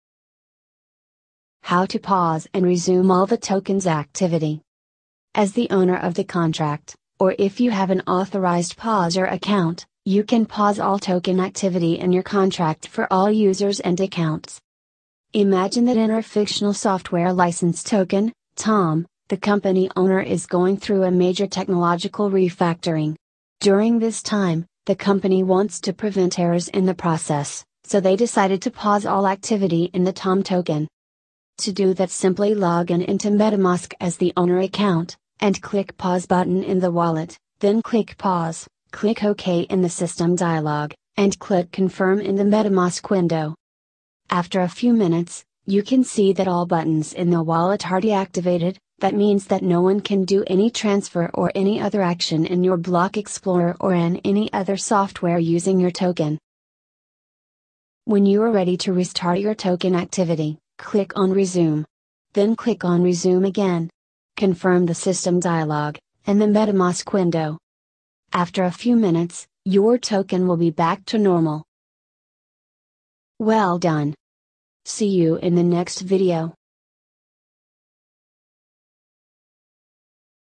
How to pause and resume all the tokens activity in ERC-20?
How to pause and resume all the tokens activity in ERC-20 using a smart contract WordPress plugin.

WPSMARTCONTRACTS - BLOCKCHAIN MADE EASY
Ethereum blockchain technology for WordPress